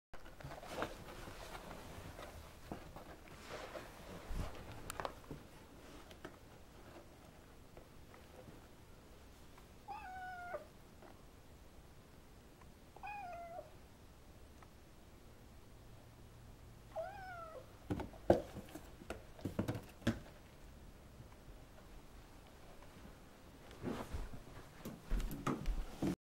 Cat Gets Head Stuck in Tissue Box - Hilariously Cute!
Mini gets her head stuck in some kleenex..shot on location in Atlanta, GA.